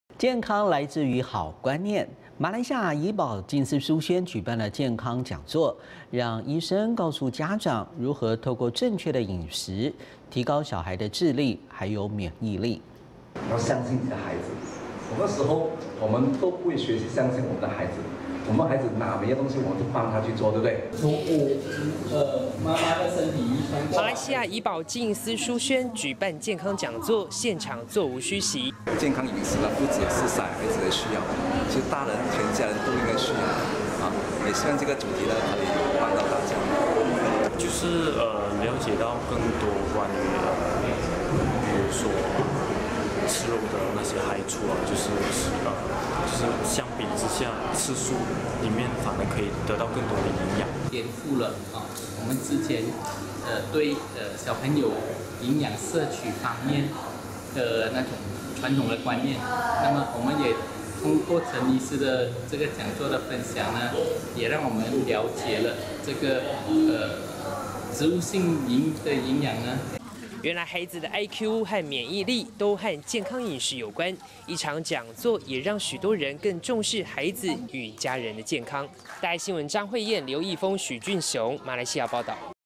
【人文】增進智力免疫力 健康飲食多留意
您相信，家中孩子的智商與免疫力，其實跟「飲食」，有高度相關嗎？馬來西亞「怡保靜思書軒」的健康講座，請來專業醫師，親自為家長解惑。(10/5)

兒科醫生 陳成亨：「要相信你的孩子，有的時候，我們都不會學習相信我們的孩子，我們孩子拿別的東西，我們都幫他去做，對不對。」

馬來西亞「怡保靜思書軒」，舉辦健康講座，現場座無虛席。

兒科醫生 陳成亨：「健康飲食不只是小孩子需要的，其實大人、全家人都應該需要的，也希望這個主題，可以幫到大家。」

民眾 庄國耀：「就是了解到更多關於，比如說吃肉的害處，就是相比之下吃素，裡面反而可以得到更多的營養。」

民眾 李榮華：「顛覆了我們之前對小朋友，營養攝取方面傳統的觀念，我們也透過陳醫師的講座分享，也讓我們了解，這個植物性的營養。」

原來孩子的IQ和免疫力，都跟健康飲食有關，一場講座，也讓許多人更重視孩子與家人的健康。

張慧艷 劉益豐 許俊雄 馬來西亞報導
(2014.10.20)